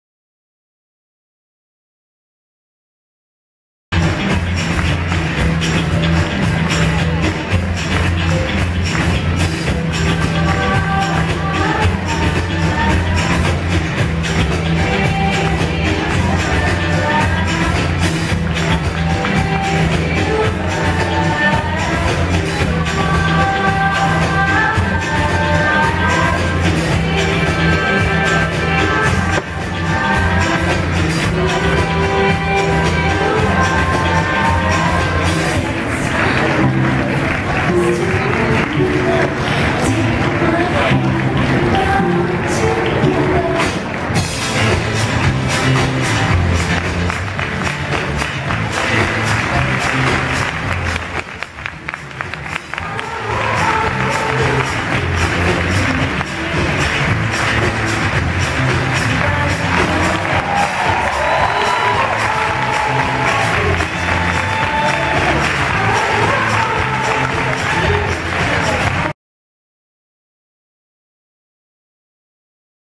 Ruffian - Spring 2013 New York Fashion Week - Meniscus Magazine
Ruffian Spring 2013 at New York Fashion Week. Our review